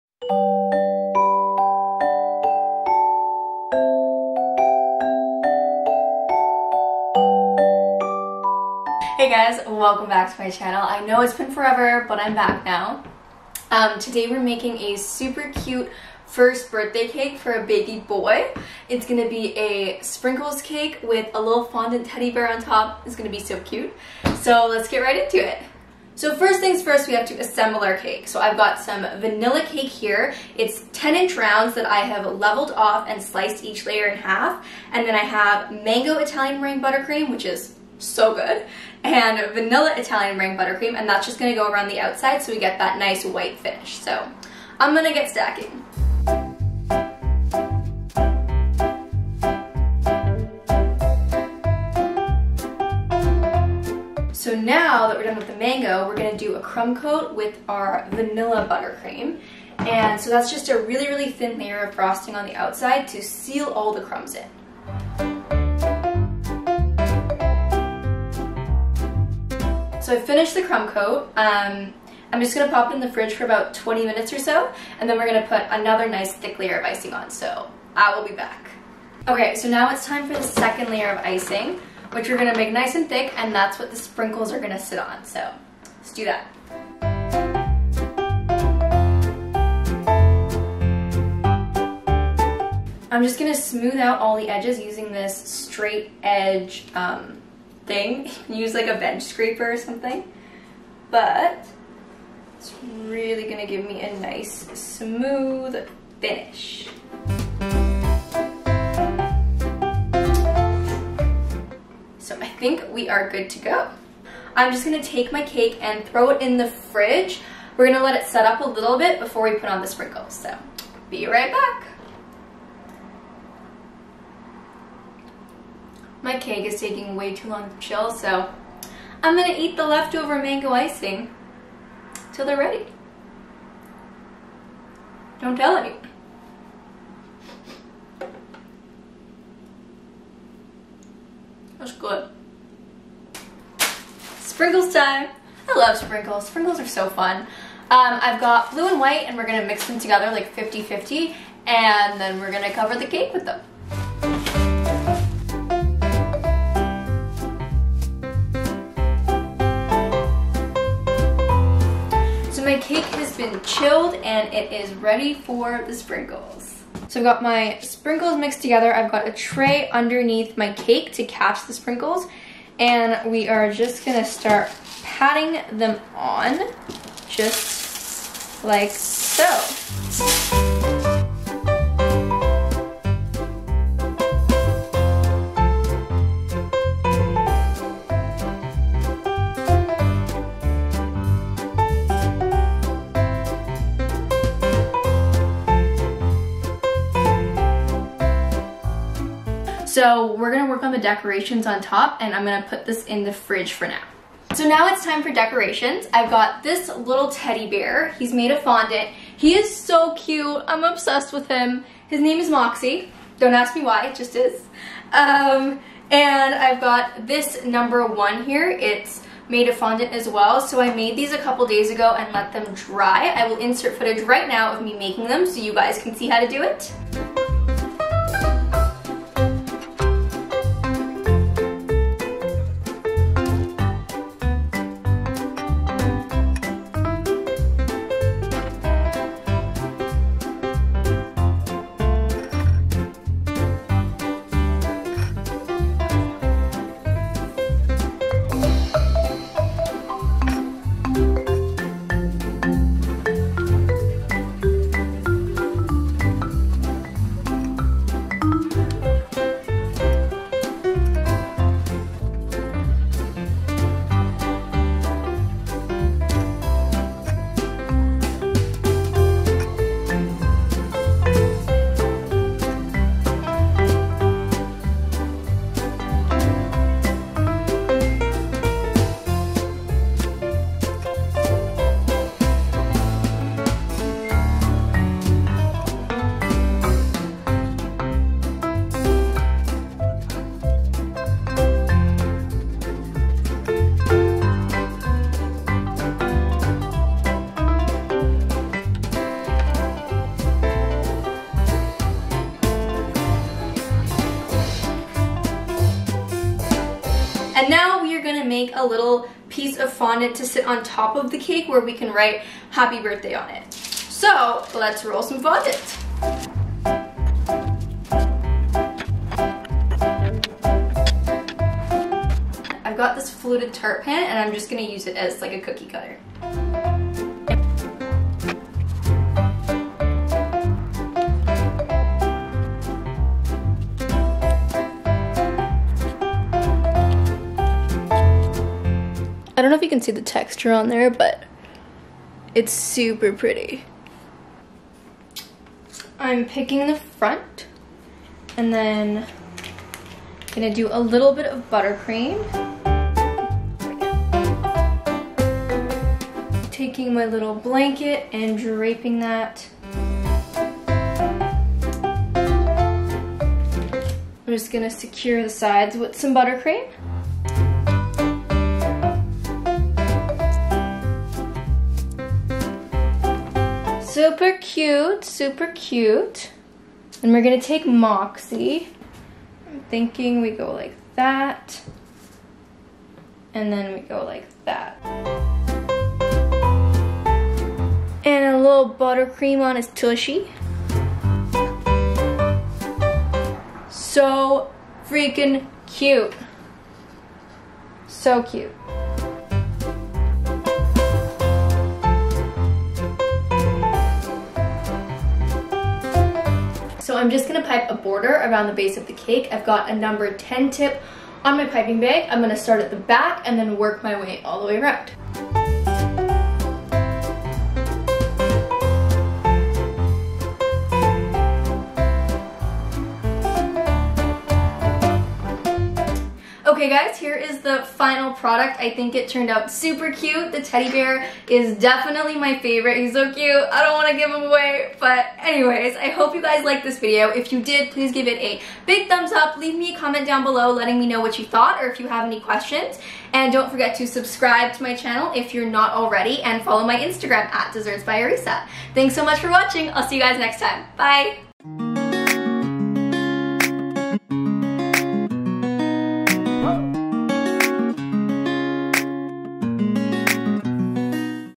Baby's First Birthday Cake | Sprinkles Cake & Fondant Teddy Bear
Learn how to make an adorable first birthday cake! Watch me as I go step by step through the process of filling and frosting a cake, covering it in sprinkles, and creating fondant decorations to go on top! Learn how to make a fondant teddy bear, number topper, and baby blanket!